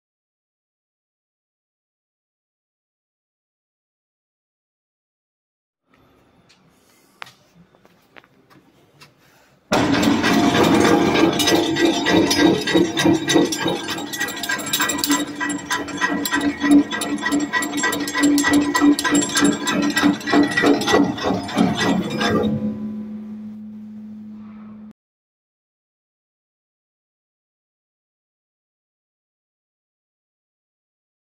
1099-74: Mado grinder
Mado grinder, model: Optimo, plate diameter: 100 mm, total dimensions: 1050 mm x 500 mm x 1380 mm.